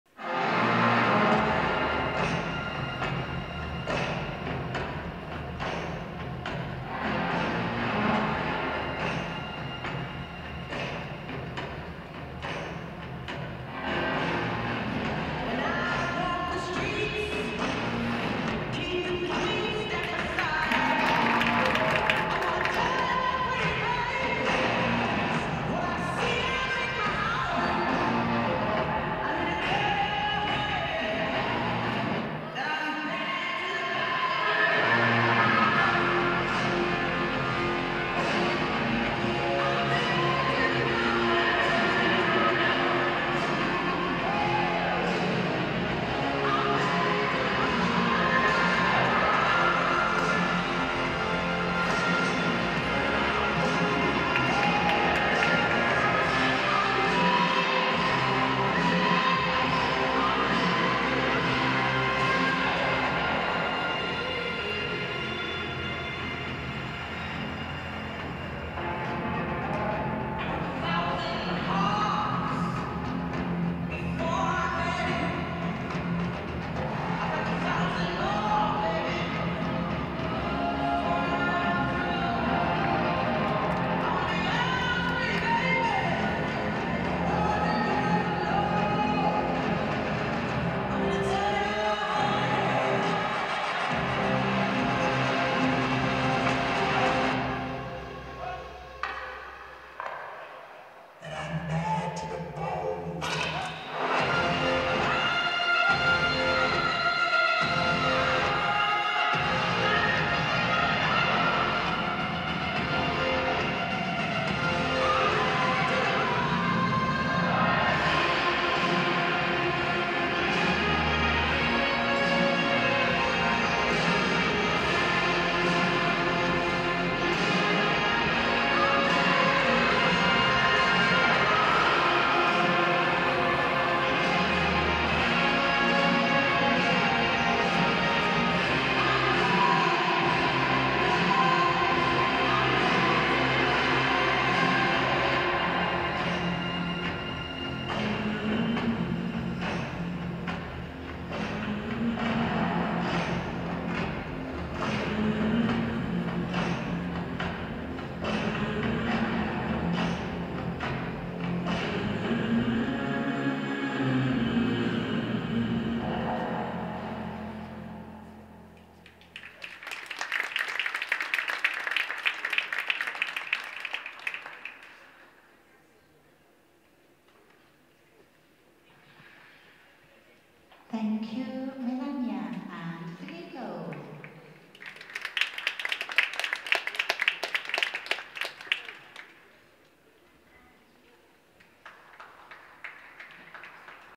4K | Milania Vaananen / Filippo Clerici (FIN) | Senior Pairs Short Program | Tayside Trophy 2023
4K | Milania Vaananen / Filippo Clerici (FIN) | Senior Pairs Short Program | Tayside Trophy 2023